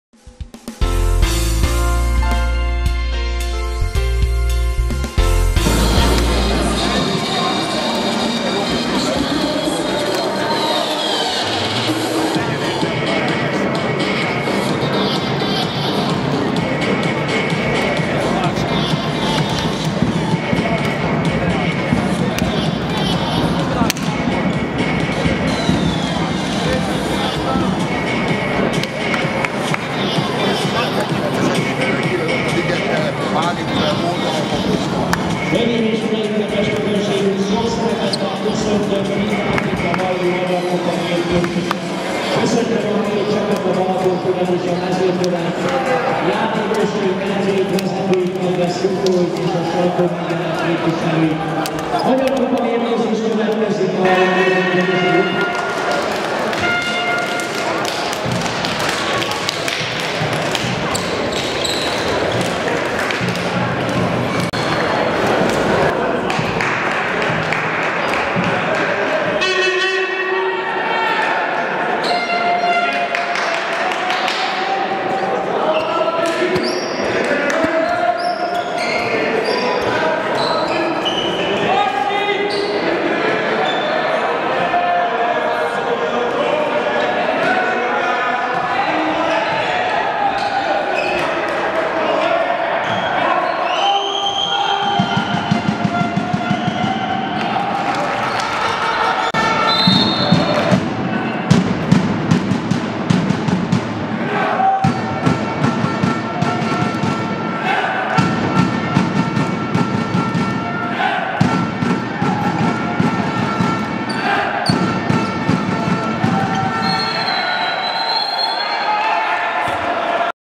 BKSE–Mezőkövesd 32:29 (15:11)
Hiába vezetett nyolc góllal, végül kiesett a Magyar Kupából kedden a balatonfüredi kézilabdacsapat. Komoly esélyt szalasztottak el.